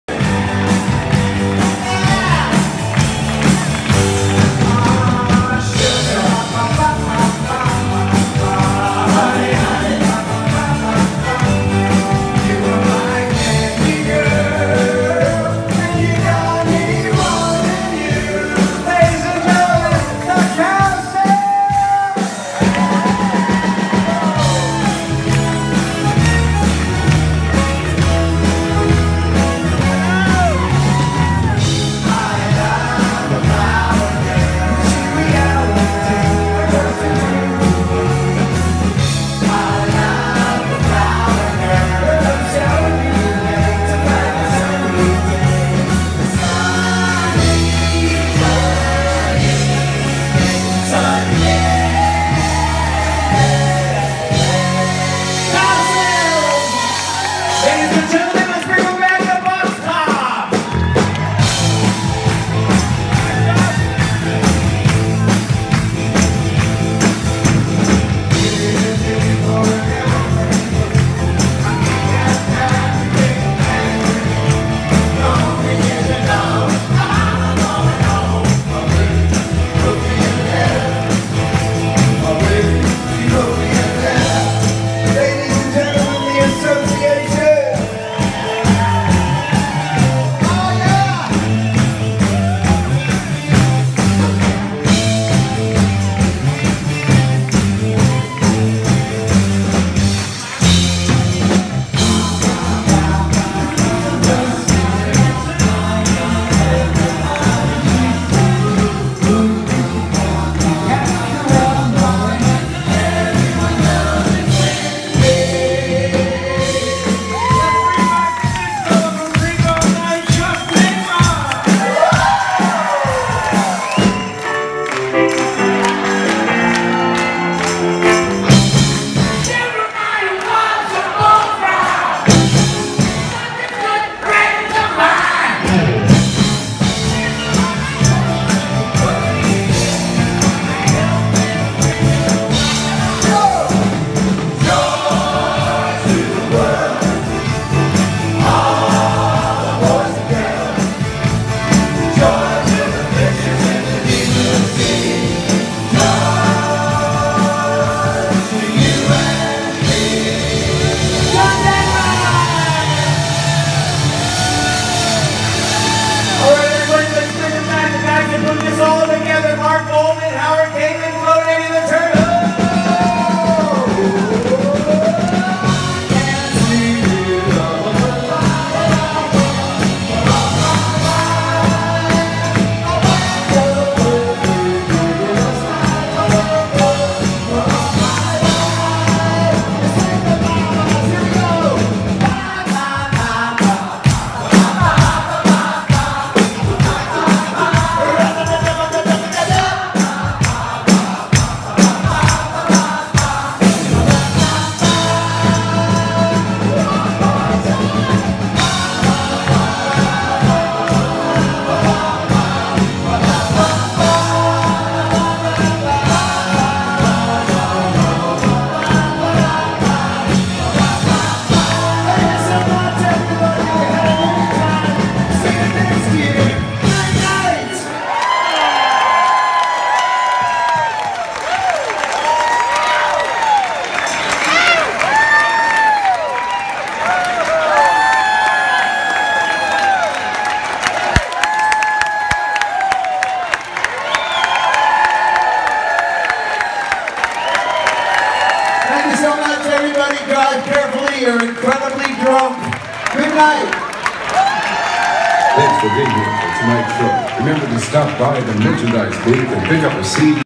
Encore will all the groups
Happy Together Tour 7/8/2017 Cache Creek Casino Brooks, CA